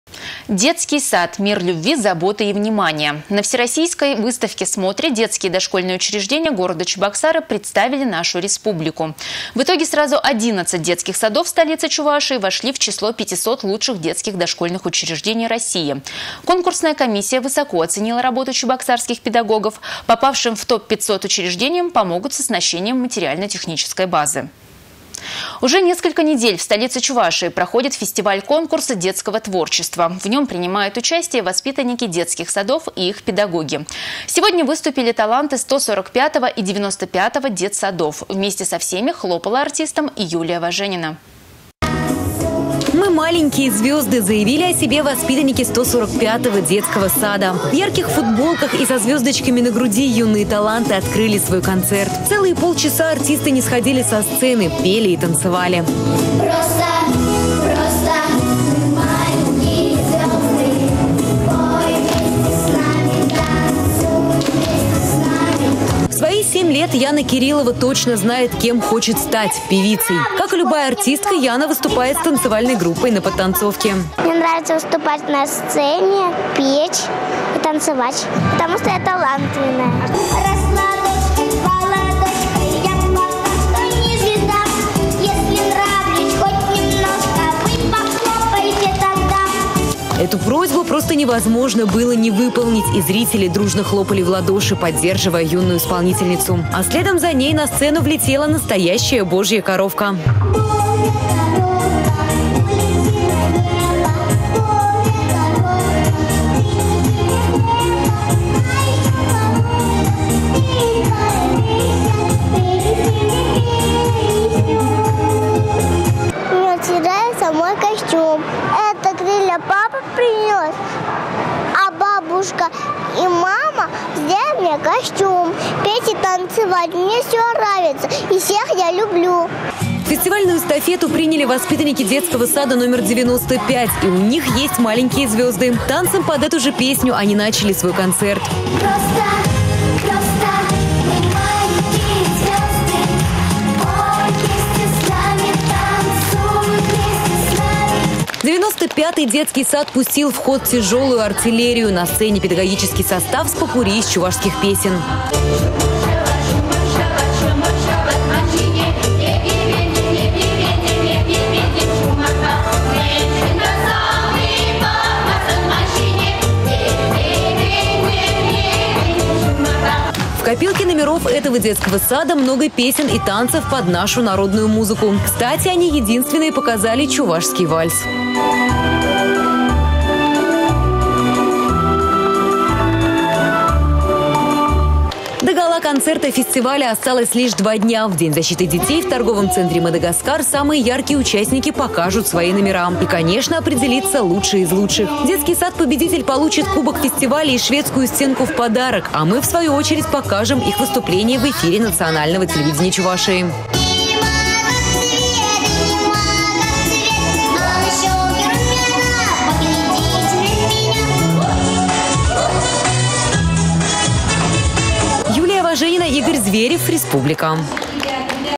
«Детский сад: мир любви, заботы и внимания!»
На всероссийской выставке-смотре детские дошкольные учреждения города Чебоксары представили нашу республику. В итоге сразу 11 детских садов столицы Чувашии вошли в число 500 лучших детских дошкольных учреждений России. Конкурсная комиссия высоко оценила работу чебоксарских педагогов. Попавшим в топ-500 учреждениям помогут с оснащением материально-технической базы.